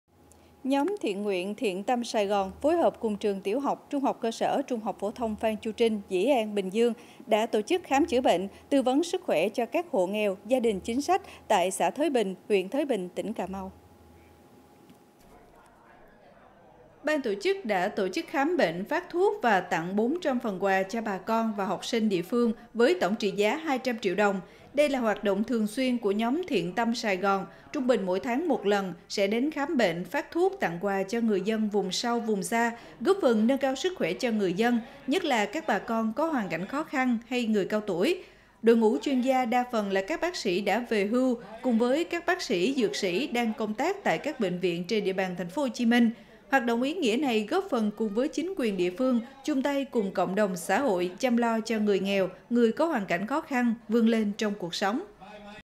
KHÁM CHỮA BỆNH, TẶNG QUÀ CHO NGƯỜI DÂN KHÓ KHĂN TẠI CÀ MAU
Nhóm Thiện Nguyện Thiện Tâm Sài Gòn phối hợp cùng Trường Tiểu học, THCS, THPT Phan Chu Trinh, Dĩ An, Bình Dương đã tổ chức khám chữa bệnh, tư vấn sức khỏe cho các hộ nghèo, gia đình chính sách tại xã Thới Bình, huyện Thới Bình, tỉnh Cà Mau.